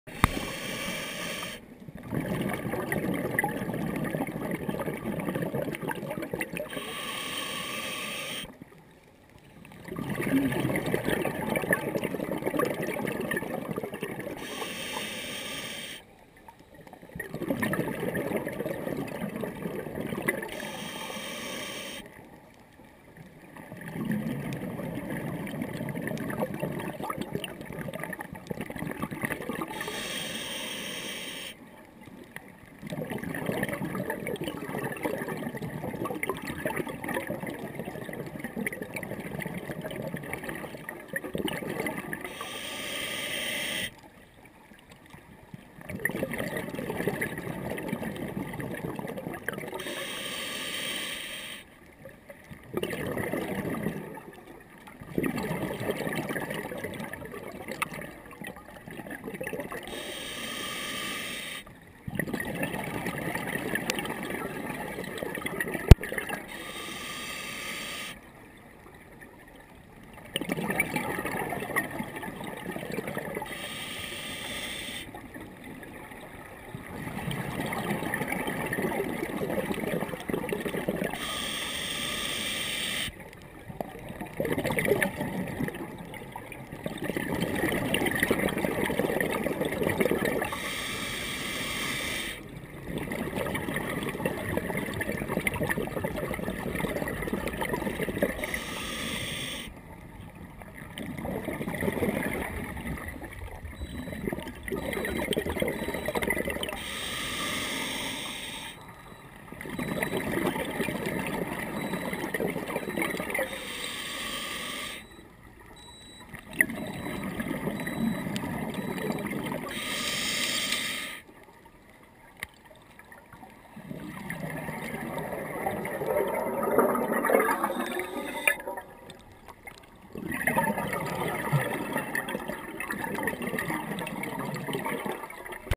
Palancar Bricks Shortest Scary swim thru
Short swim thru in at 70 feet turn 180 deg and then back out at 92 feet,